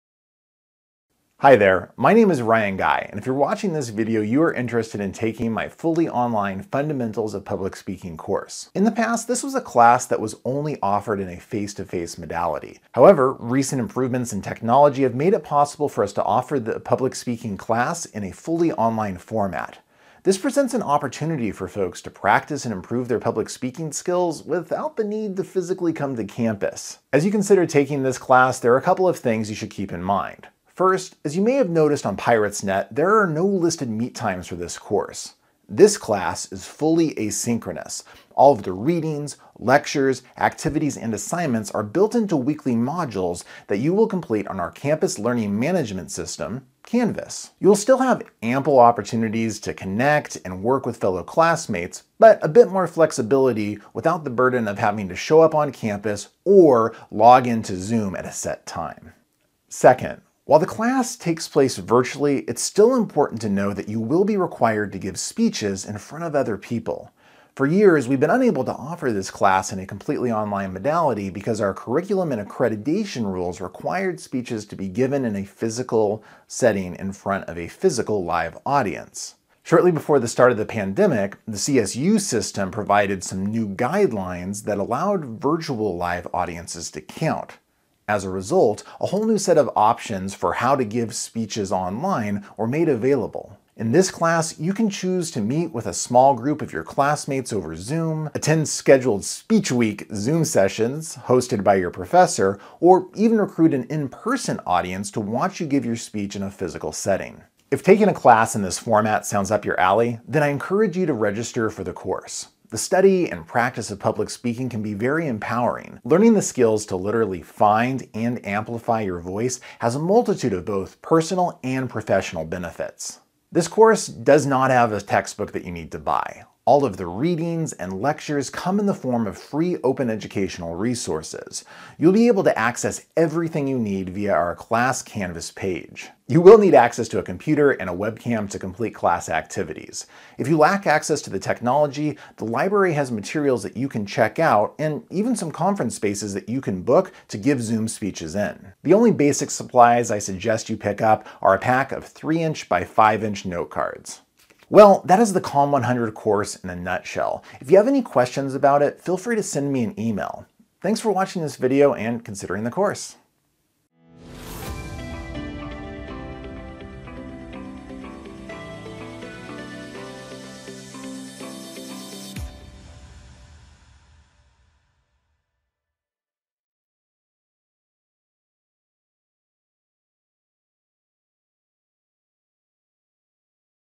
COMM 100 Overview (Asynchronous Course at Modesto Junior College)
to register for this class.

Hey there! My name is Ryan Guy and if you are watching this video you are interested in taking my Fully Online "fundamentals of public speaking" course.

In the past this was a class that was only offered in a face to face modality. However, Recent improvements in technology  have made it possible for us to offer this public speaking classes in fully online formats. This presents an opportunity for folks to practice and improve their public speaking skills, without the need to physically come to campus. As you consider taking this class there are a couple of things you should keep in mind.

First, as you may have noticed on PiratesNet, there are no listed meet times for this course. This class is fully asynchronous. All readings, lecturers, activities and assignments are built into weekly modules that you will complete on our campus Learning management system: Canvas. You still will have ample opportunities to connect and work with fellow classmates, but with a bit more flexibility and without the burden of having to show up on campus or login to Zoom at a set time.

Second, while the class does takes place virtually, it is still important to know that you will be required to give speeches in front of other people. For years we have been unable to offer this class in a completely online modality because our curriculum and accreditation rules required speeches to be given to a physical   "live audience."  Shortly before the start of the pandemic the CSU system provided some new guidance that allowed "virtual live audiences" to count.  As a result a new set of options for how to give a speech online were made available.  In this class you can choose to meet with a small group of classmates over zoom, attend scheduled “speech week” Zoom sessions hosted by your professor, or even recruit an in person audience to watch you give your speech in a physical setting.

If taking a class in this format sounds up your alley, then I encourage you to register for the course. The study and practice of public speaking can be very empowering. Learning the skills to literally find and amplify your voice has a multitude of both personal and professional benefits.

This course does not have a textbook you need to buy. All the readings and lectures come in the form of free open educational resources. You will be able to access everything you need via our class Canvas page. You will need access to a computer and a webcam to complete class activities.  If you lack the technology, the library has materials you can check out and even some conference spaces you can book to give Zoom speeches in. The only basic supplies I suggest you pick up are a pack of 3 inch by 5 inch note cards.

Well that is the online comm 100 course in a nutshell.